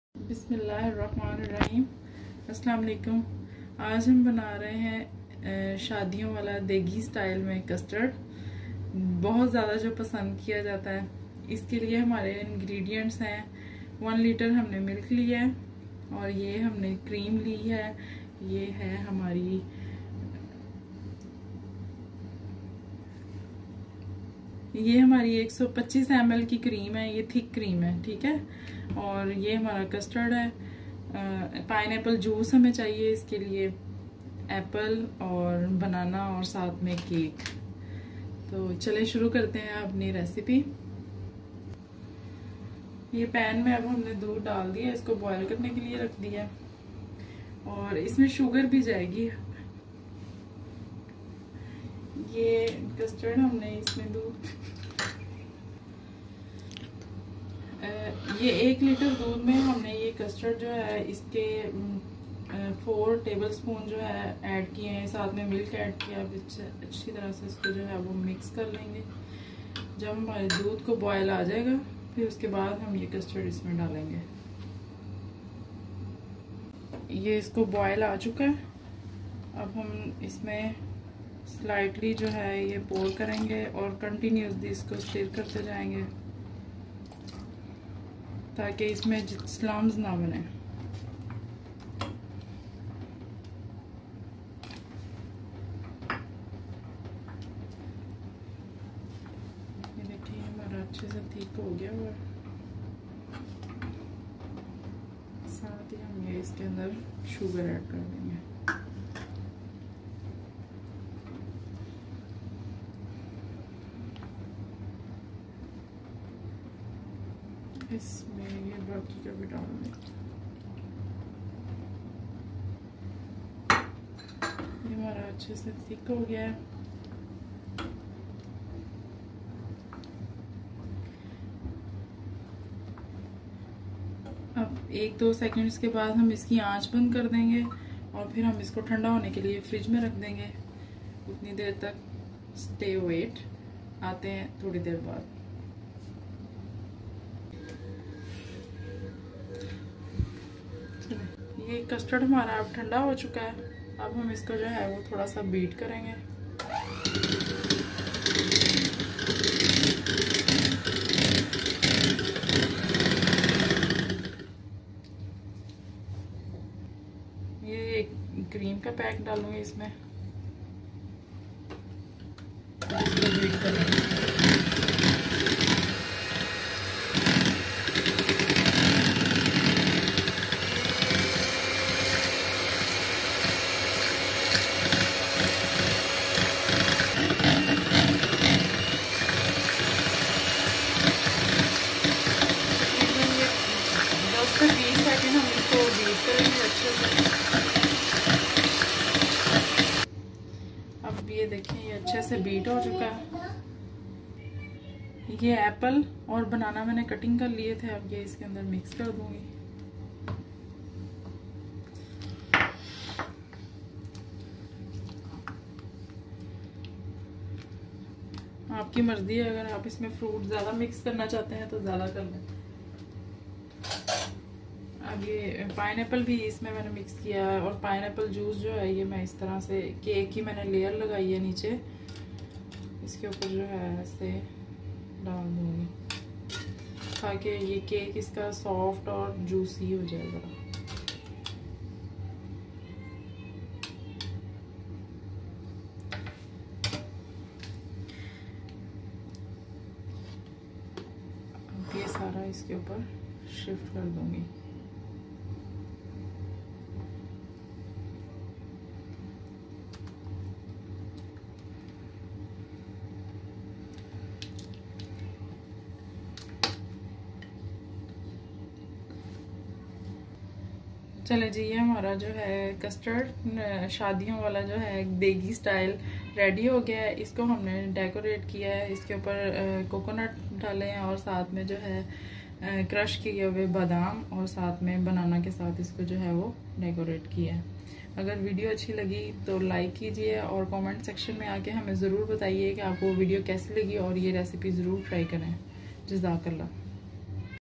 Fruit Custard Recipe|Shadion wala Degi Custard|Creamy Easy Dessert
It a new style n easy recipe of fruit creamy custard.Its taste is like a Degi custard.very delicious taste. I hope you will enjoy this recipe.very easy & quick recipe.

Ingredients
Milk 1 liter
Foster clark's (custard powder) 4 tab spoon
Banana,Apple, pineapple, Cake
Pineapple juice, crushed almonds, crushed coconut.
Sugar 1 cup
Thick Cream 125 ml